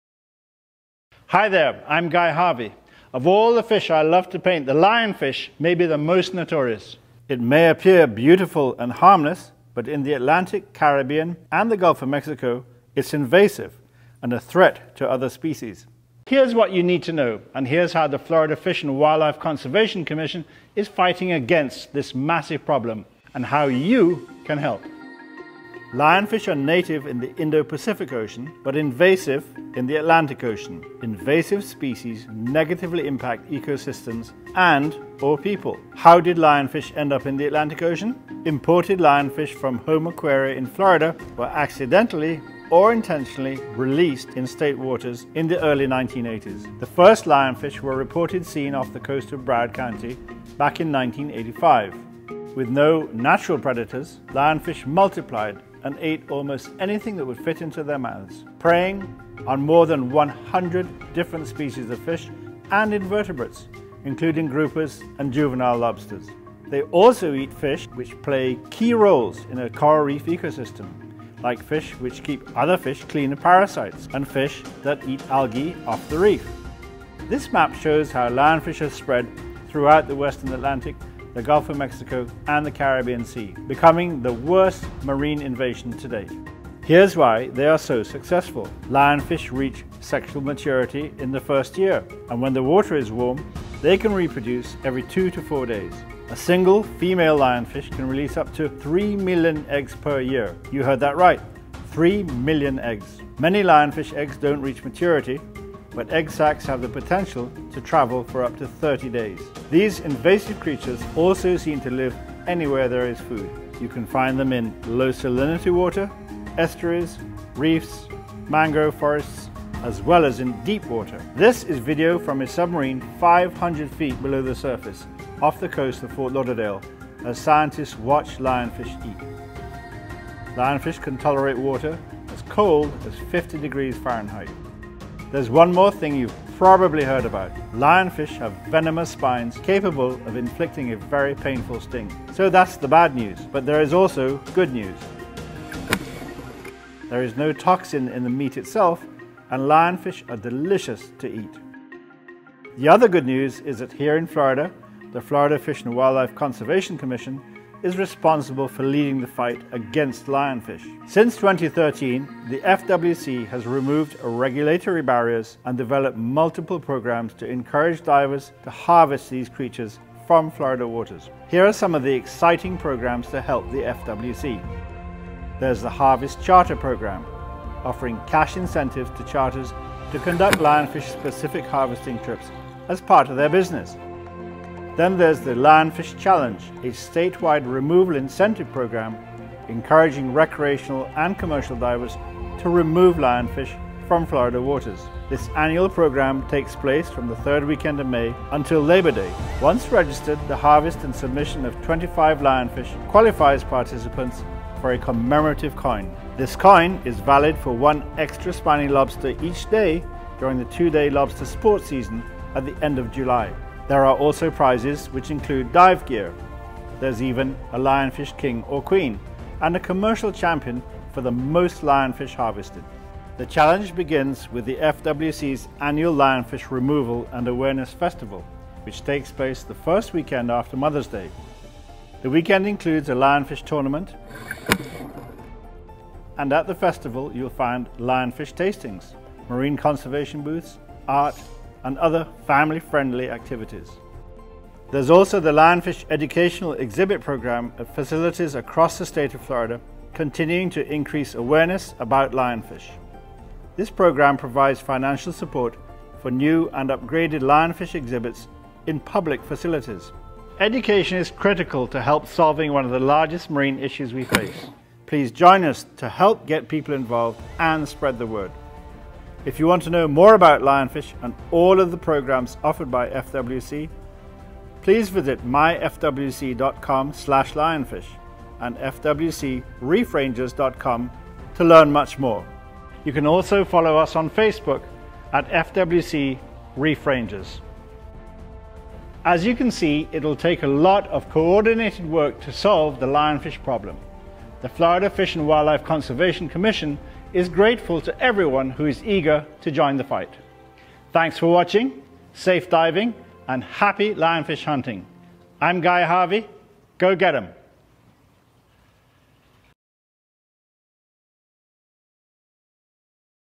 Guy Harvey: Lionfish Invasion
Conservationist and artist Guy Harvey narrates the current status of the lionfish invasion and ways you can help protect Florida's ecosystem through Florida Fish and Wildlife Conservation Commission programs.